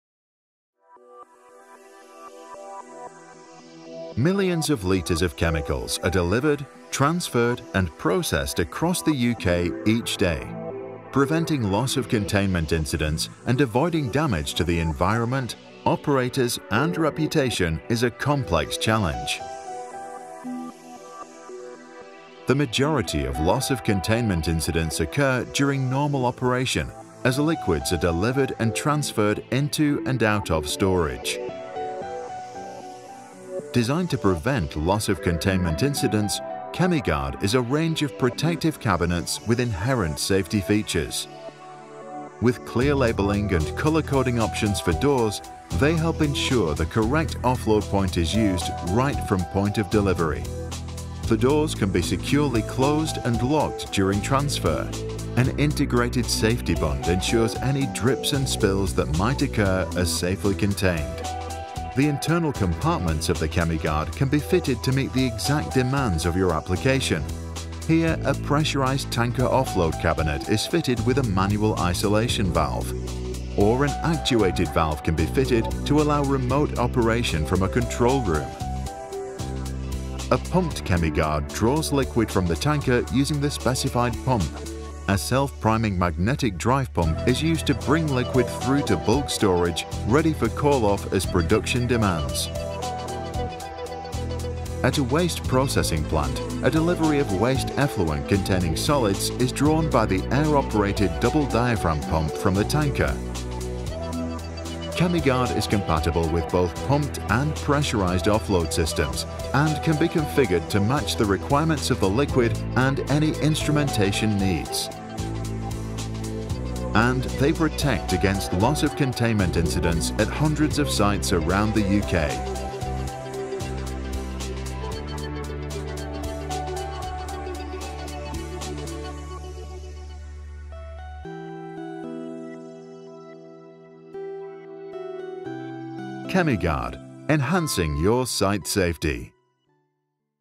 ChemiGuard Tanker Offload Cabinets
ChemiGuard Pressurised and Pumped Offload Cabinets protect operators and the environment from Loss of Containment incidents. The internal compartments can be configured to include pipework, valves and instrumentation required for the process liquid and specific application